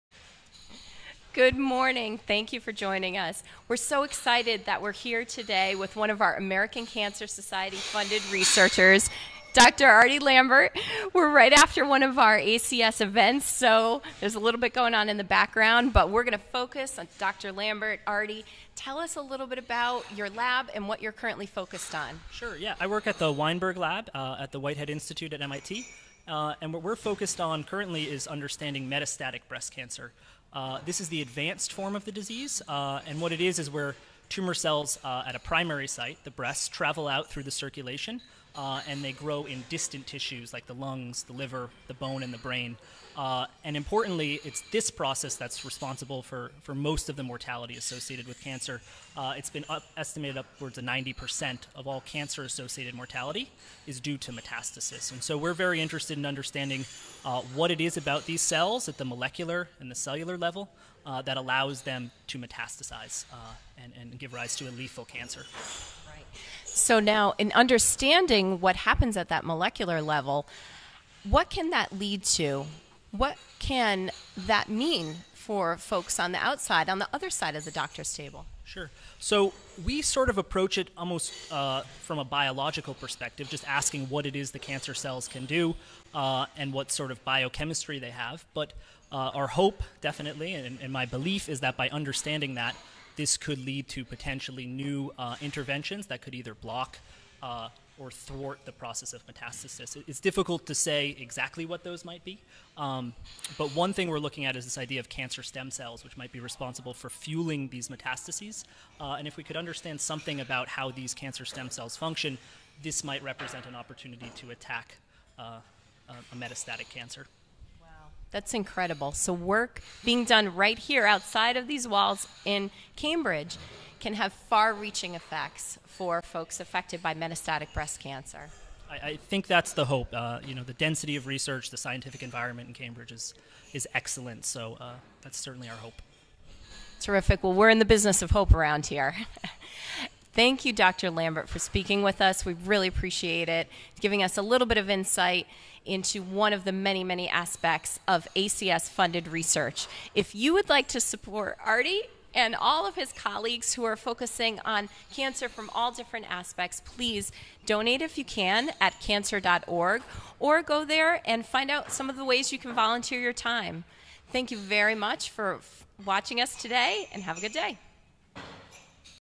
Dr. Arthur W. Lambert, American Cancer Society researcher
American Cancer Society-funded researcher Dr. Arthur W. Lambert discusses his current work focusing on stem cells, tumor cell dormancy and metastatic breast cancer colonization after the ACS CAN research Breakfast on June 12, 2017 in Boston, Mass.